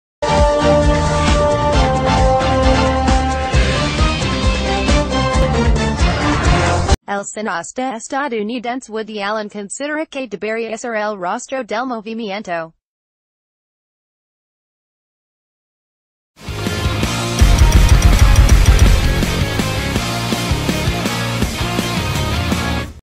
Espectaculos | La Crónica de Hoy
El cineasta estadunidense Woody Allen considera que debería ser el rostro del movimiento MeToo (YoTambién), por el que decenas de actrices denunciaron haber sido víctimas de abusos sexuales en el seno de la industria cinematográfica de Estados Unidos.